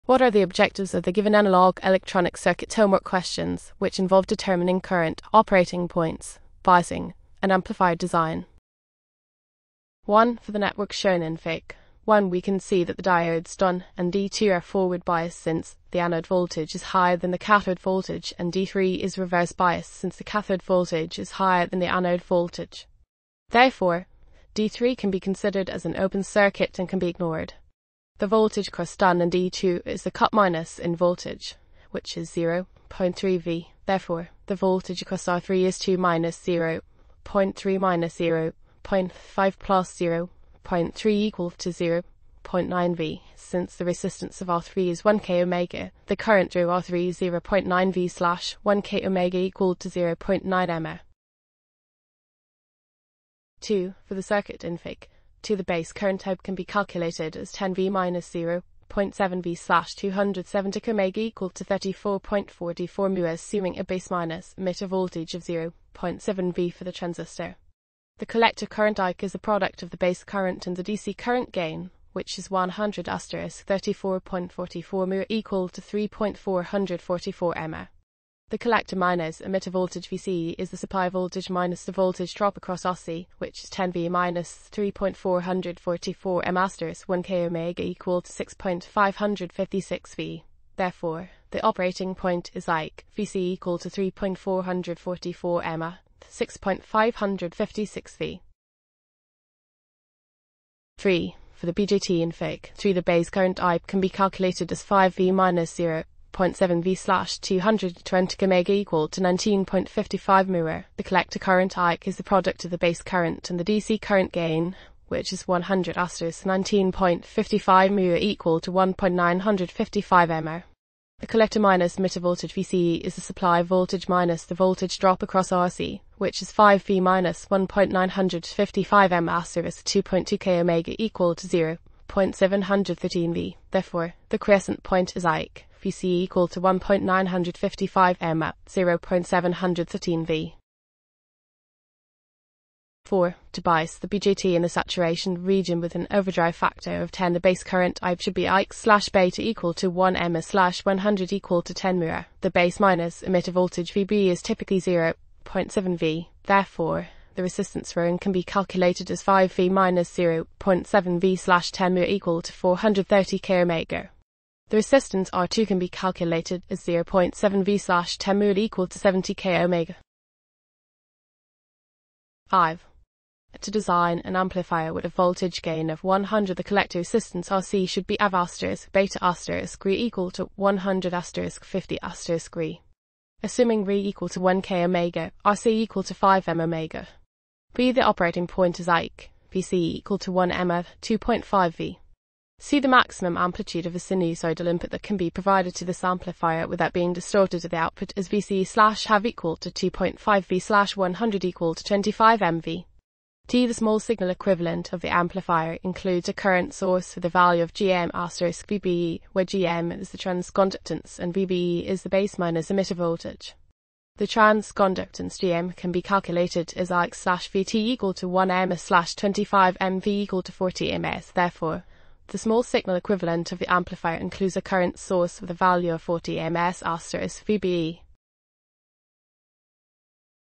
Analog Electronic CircuitsHomework Sheet 041 Consider the network shown in Fig 1 Diodes D to D3 a
Analog Electronic Circuits Homework Sheet 04 1. Consider the network shown in Fig. 1. Diodes D: to D3 are identical, have a cut-in voltage of 0.3 V and are ideal otherwise. The terminals A, B and C are found to be at 2 V, 0.5 V and -2 V respectively If all the resistors are of 1 k, determine the current through R3. 2. For the circuit in Fig. 2, the transistor Ti has a dc current gain of 100. If the voltage supply is 10 V, R, = 270 k; R = 30 k and Rc = 1 k, determine the operating point (Ic,VcE) 3. The BJT of Fig. 3 has a dc current gain of 100. The biasing supply is of 5 V. If R,R and R3 are 220 k, 2.2 k and 100 respectively, determine the quiescent point. 4. Given a BJT with a = 100, bias it using a voltage divider such that it is in the saturation region with an overdrive factor of 10, to deliver a collector current of 1 mA. You are provided with a voltage supply of 5 V. 5. (a) Given a single power supply of 5 V and a BJT with a dc current gain of 50, design an amplifier with a voltage gain of 100. The quiescent current should be limited to 1 mA; (b) What is its operating point; (c) What is the maximum amplitude of a sinusoidal input that can be provided to this amplifier, without being distorted at the output ? (d) Determine the small signal equivalent of the amplifier. Vce R1 A W R1 D1 4I D D2 K B W R2 R c Fig.1 Fig. 2 Fig. 3